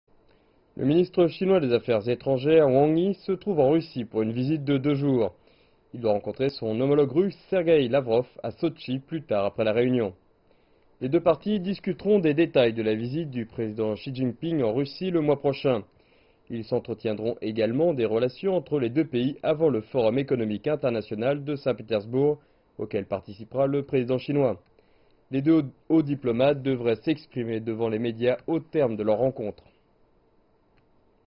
Le ministre chinois des Affaires étrangères se trouve en Russie pour une visite de deux jours
Le ministre chinois des Affaires étrangères, Wang Yi, se trouve en Russie pour une visite de deux jours